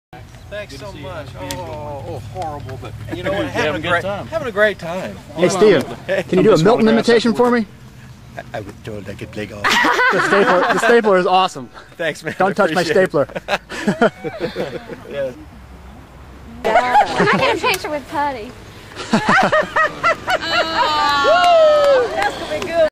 Milton from Office Space Stephen Root
Stephen Root from office space does his Milton imitation at a celebrity golf tournament in Winston Salem NC.